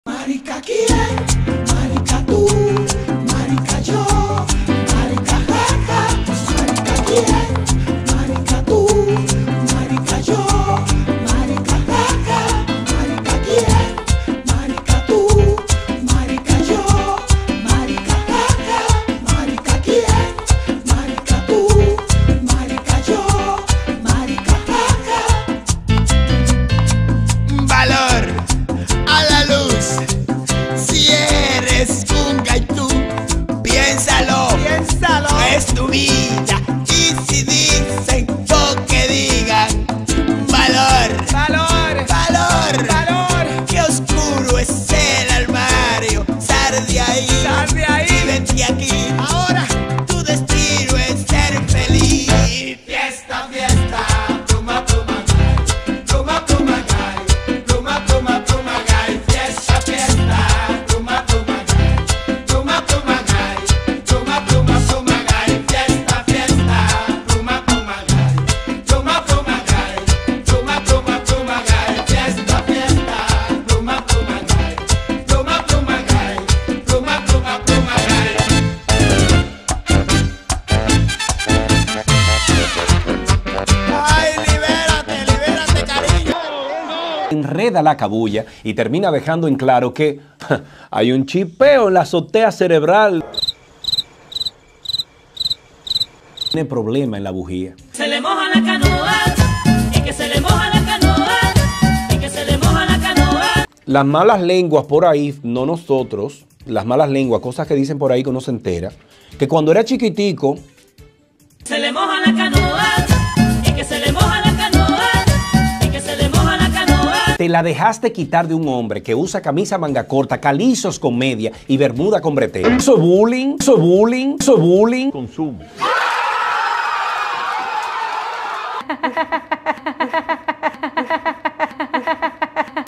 SERGIO CARLOS SALDRA DEL CLOSET
Que me digan maricon no me ofende y no debe ofender a nadie de la comunidad LGBTQ…Sergio Carlo sobre designación de sobrina en gobierno: «Sí mete la pata, que se prepare». como vicecónsul de República Dominicana en Valencia, España.